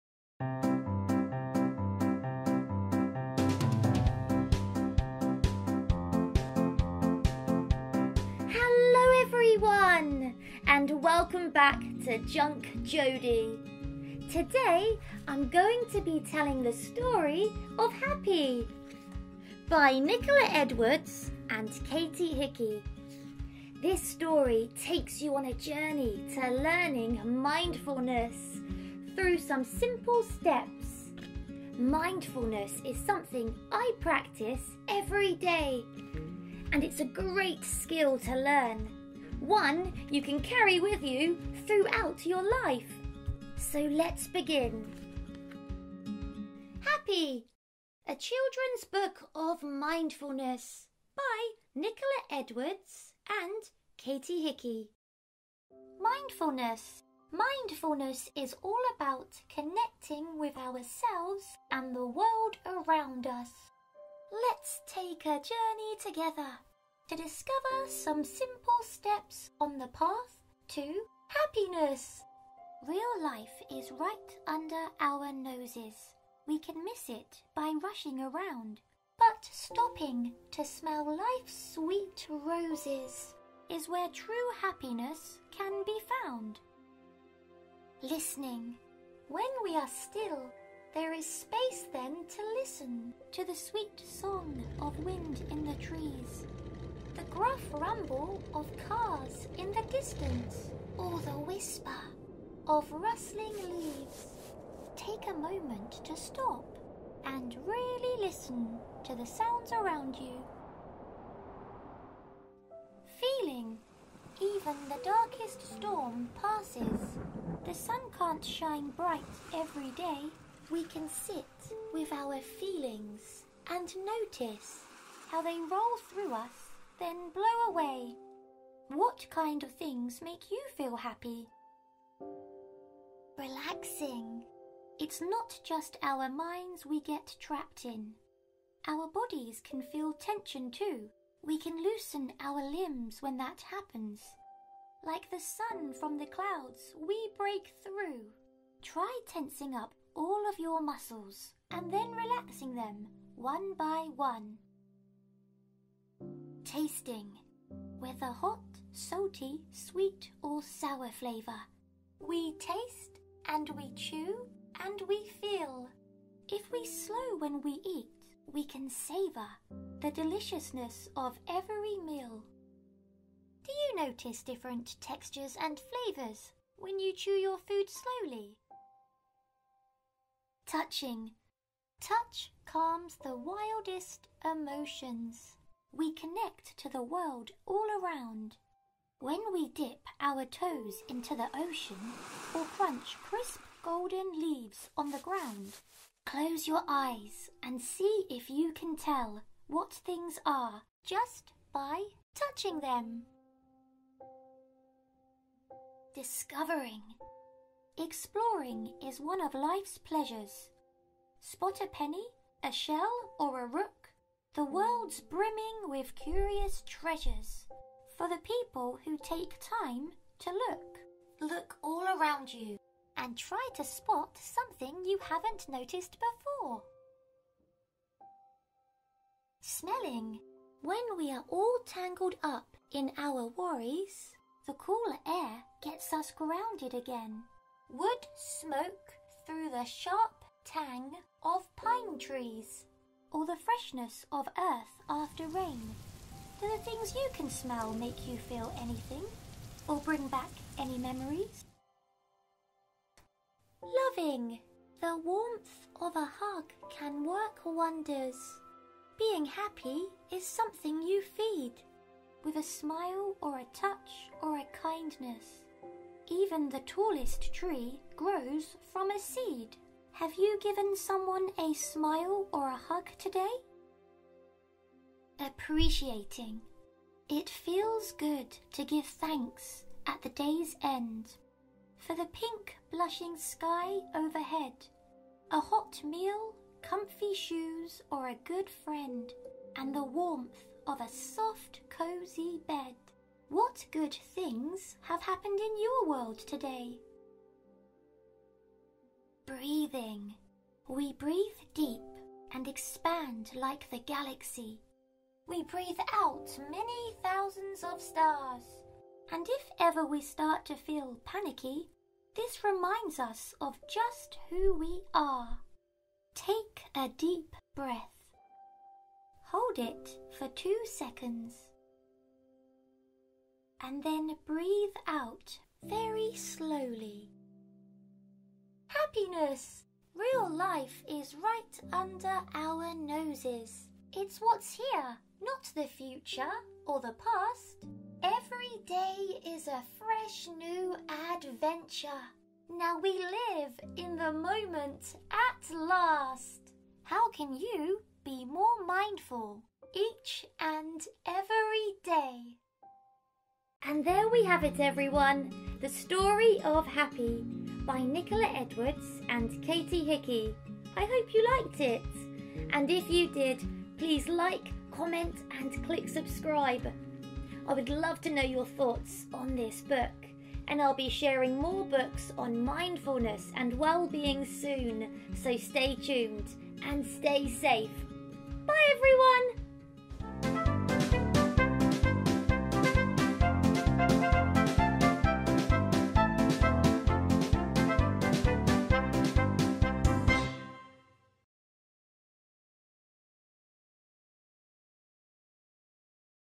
Happy book - Nicola Edwards | A Children's Book of Mindfulness | Children's Well-being | Story-time
Hi Everyone,

For this episode I am telling the story of 'Happy' by Nicola Edwards and Katie Hickey. This story takes you on a journey to discover some simple steps to understanding mindfulness.

Author: Nicola Edwards
Illustrator: Katie Hickey
Publisher: Caterpillar Books Ltd an imprint of Little Tiger Press Group
You can purchase from: Amazon